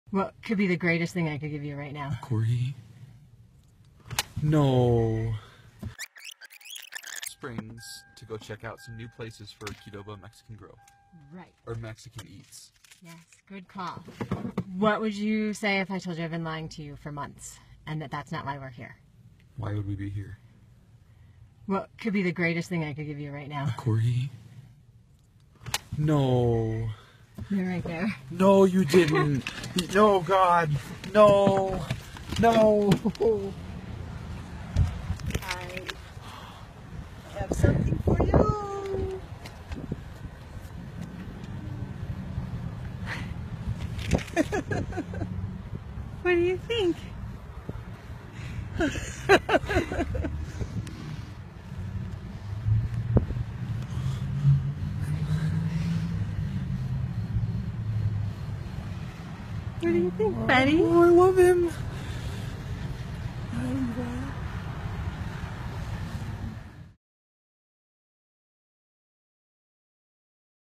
Kid Reacts to Corgi Puppy Surprise | The Dodo
Kid Reacts to Surprise Corgi Puppy | He's been waiting SO LONG for this moment ❤️

Video by: Maggie McCarthy